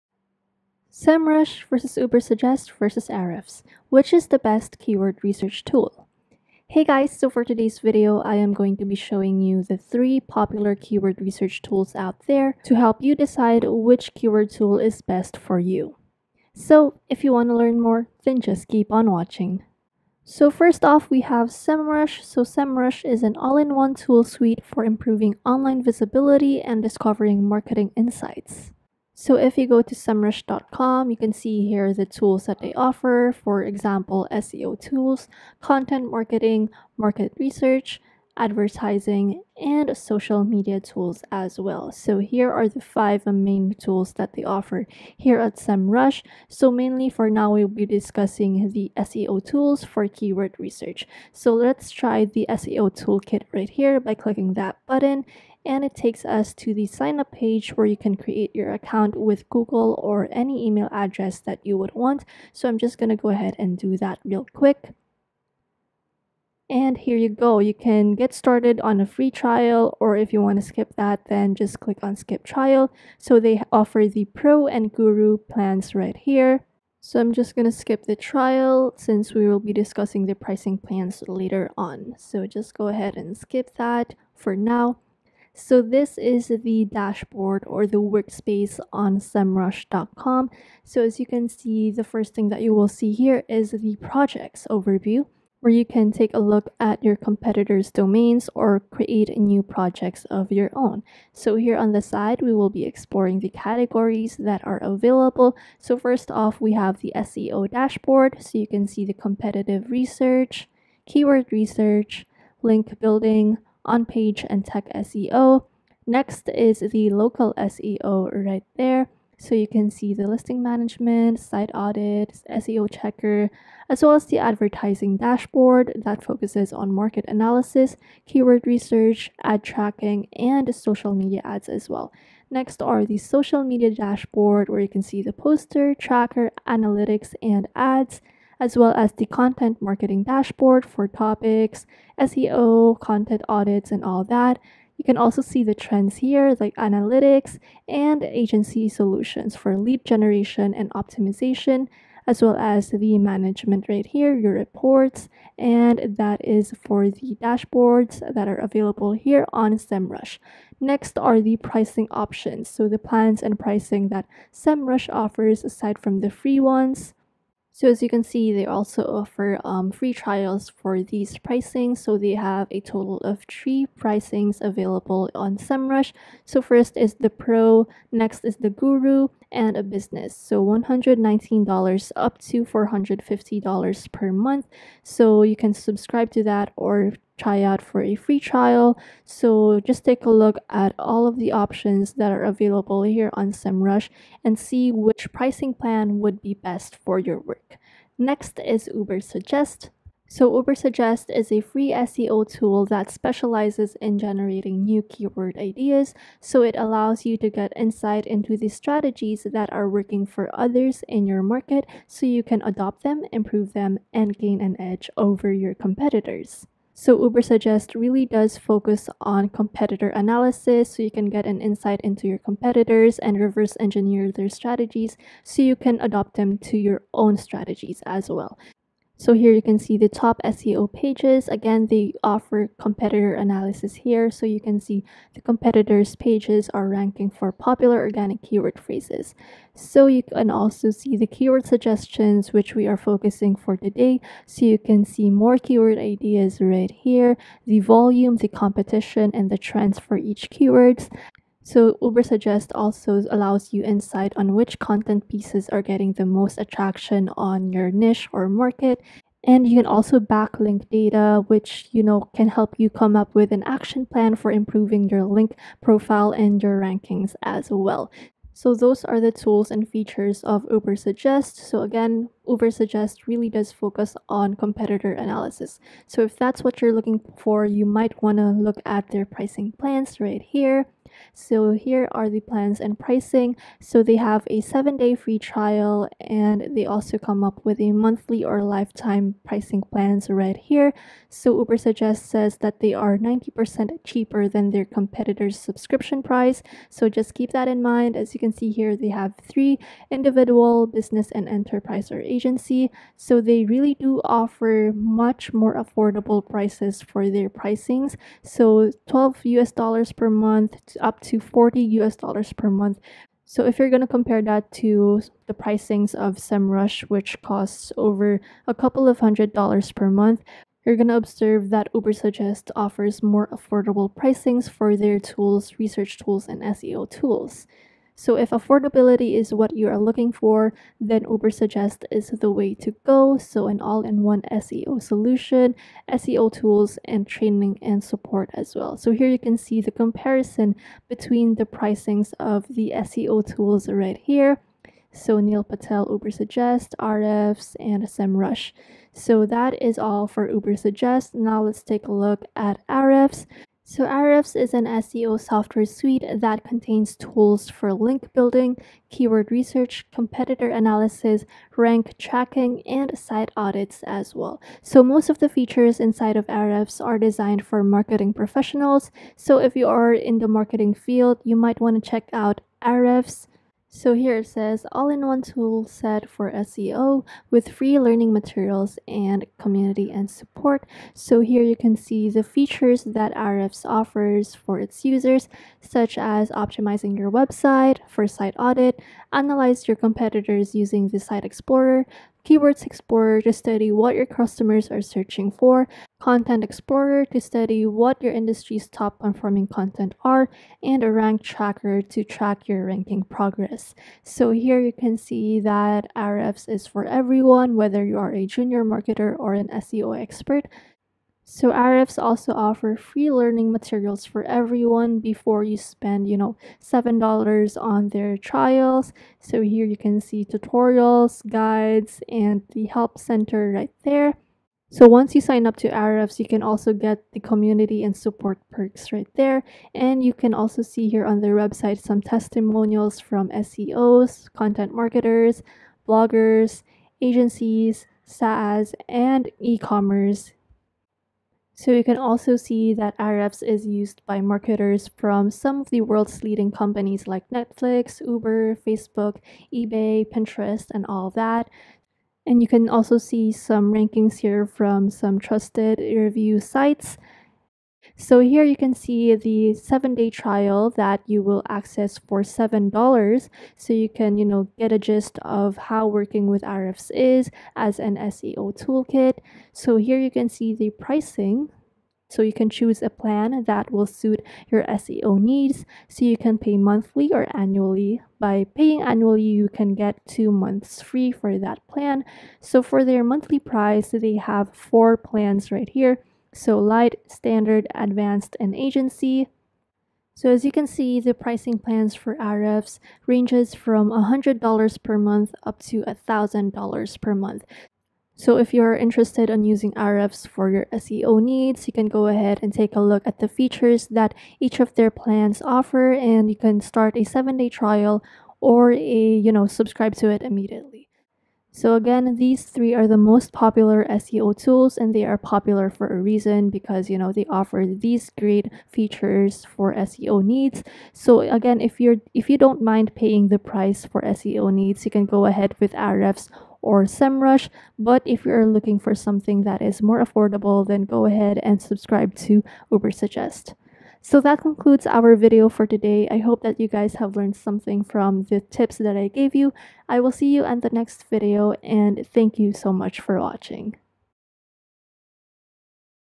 Semrush vs Ubersuggest vs Ahrefs | Best Keyword Research Tool
Learn Semrush vs Ubersuggest vs Ahrefs | Best Keyword Research Tool

In this video, I will compare three keyword research tools namely Semrush, Ubersuggest, and Ahrefs. This video guide will discuss each platform’s pricing plans and features such as generating content outlines with AI, keyword rank tracking, accurate ranking difficulty, exportable keyword data, and more to boost your SEO.